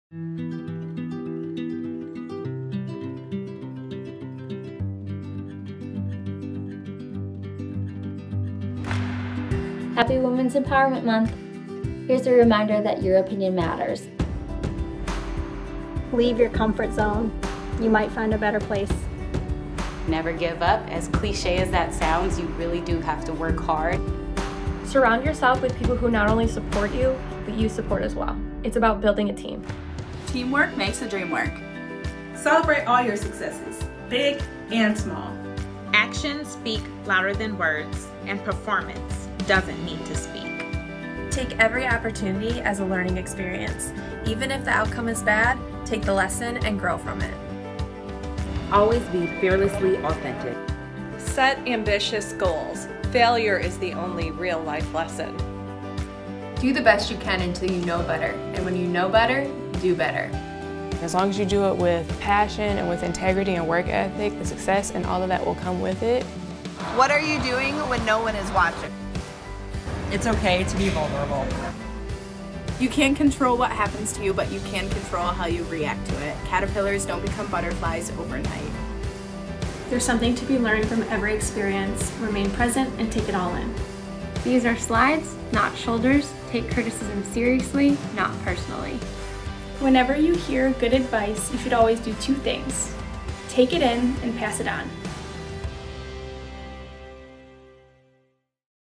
International Women's Day 2021
𝔅𝔲𝔦𝔩𝔡 𝔞 𝔱𝔢𝔞𝔪.
𝒞𝑒𝓁𝑒𝒷𝓇𝒶𝓉𝑒 𝓎𝑜𝓊𝓇 𝓈𝓊𝒸𝒸𝑒𝓈𝓈𝑒𝓈.
𝕀𝕥’𝕤 𝕠𝕜𝕒𝕪 𝕥𝕠 𝕓𝕖 𝕧𝕦𝕝𝕟𝕖𝕣𝕒𝕓𝕝𝕖.

Women of the Detroit Pistons share advice they’ve picked up on throughout their careers and pass it on to the next generation of women leaders on International Women's Day 2021.